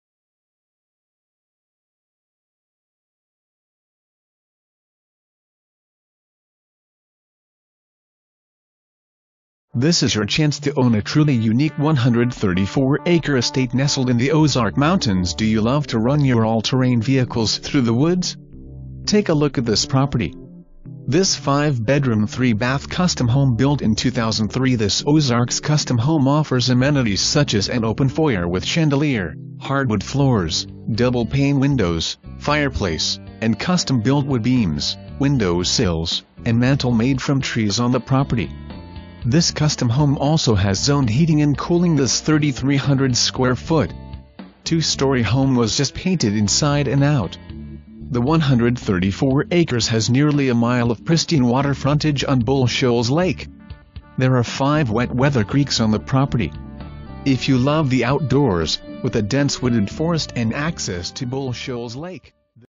Protem MO 134 wooded acres with custom home For Sale
2500 Elbow Road
Protem, MO 65733

This is your chance to own a truly unique 134-acre estate nestled in the Ozark Mountains. This 5 bedroom 3 bath custom home built in 2003.The home offers amenities such as an open foyer with chandelier, hardwood floors,double pane windows,fireplace,and custom built wood beams,window sills,and mantle made from trees on property.Home also has zoned heating and cooling This 3300 sq. ft. two story home was just painted inside and out.This land has nearly a mile of pristine water frontage on Bull Shoals Lake.

Presented By:
Carolyn Crispin, Keller Williams Tri Lakes
View My Inventory:
Branson MO real estate for sale keller Williams realty